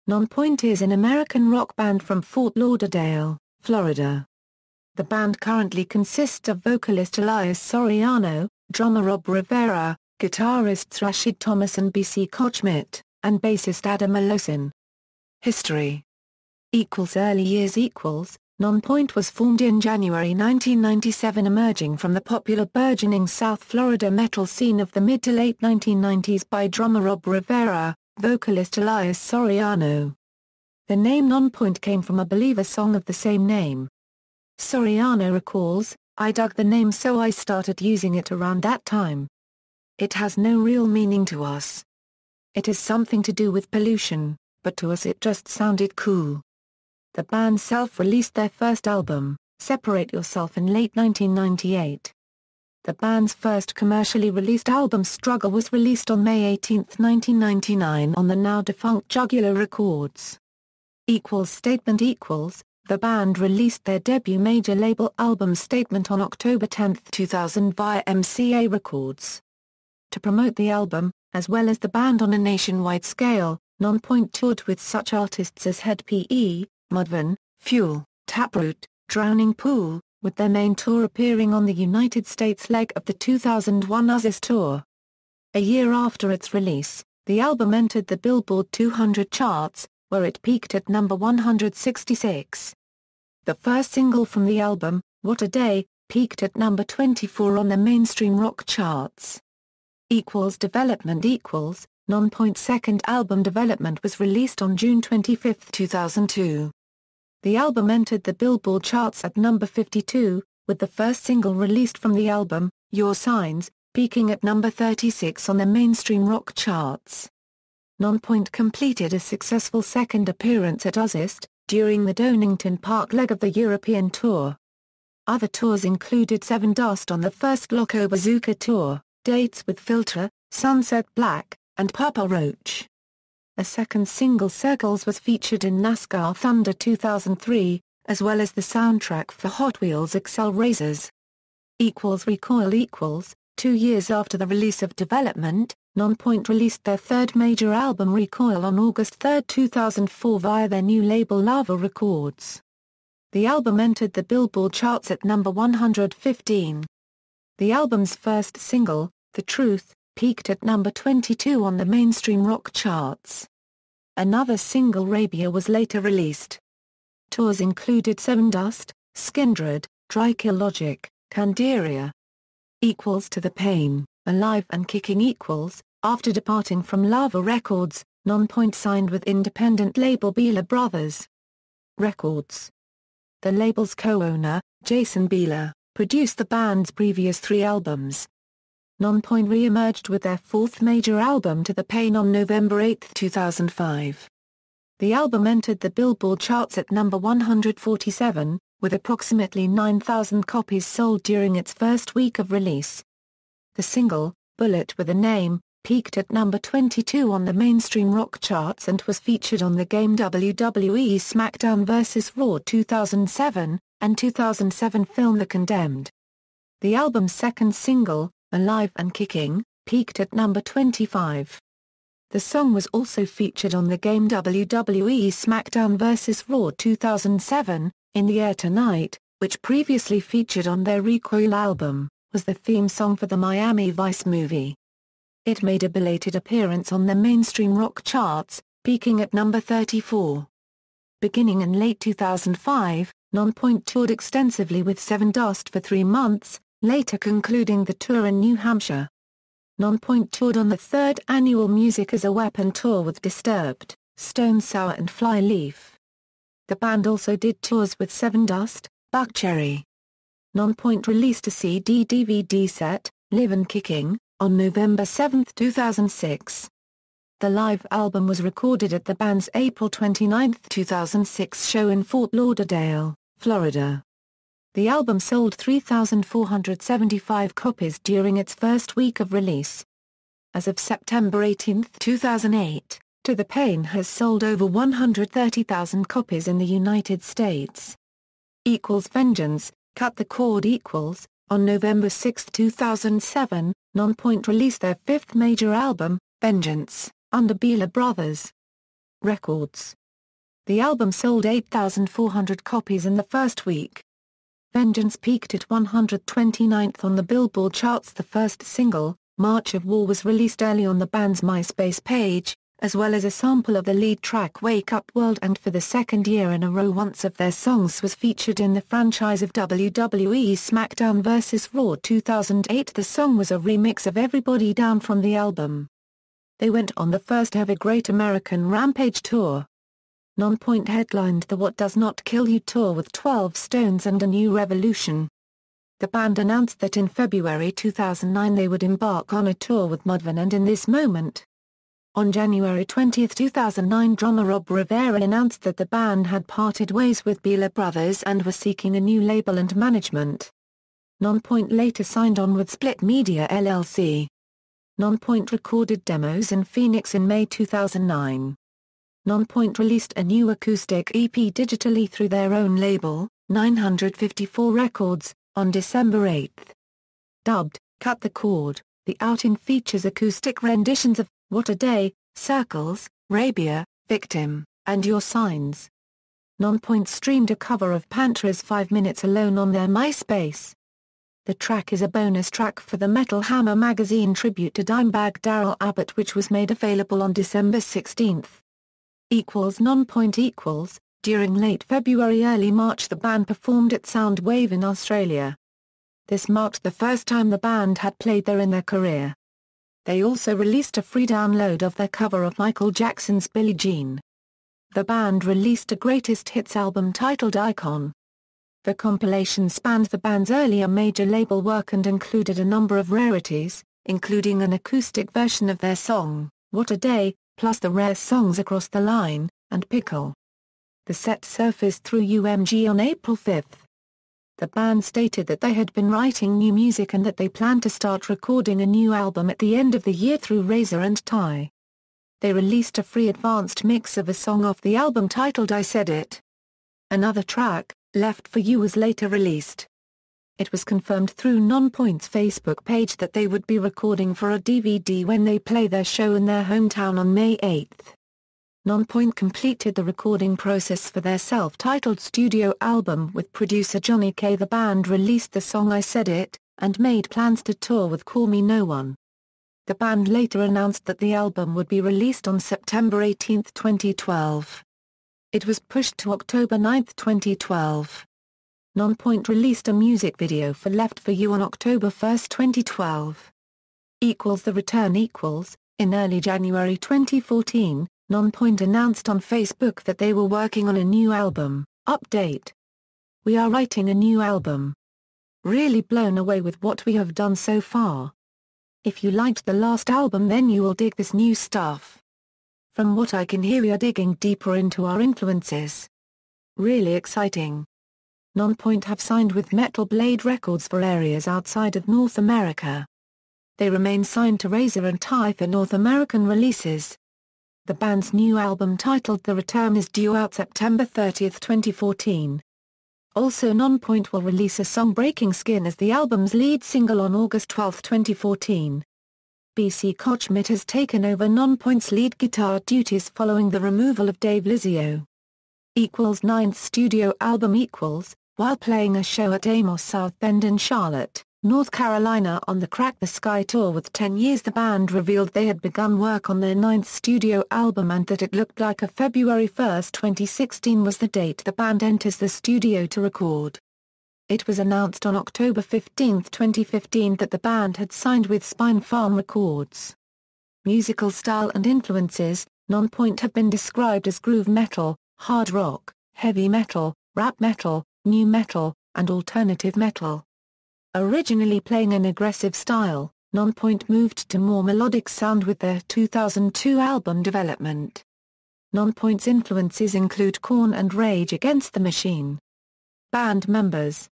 Nonpoint
Nonpoint is an American rock band from Fort Lauderdale, Florida. The band currently consists of vocalist Elias Soriano, drummer Robb Rivera, guitarists Rasheed Thomas and B.C. Kochmit, and bassist Adam Woloszyn.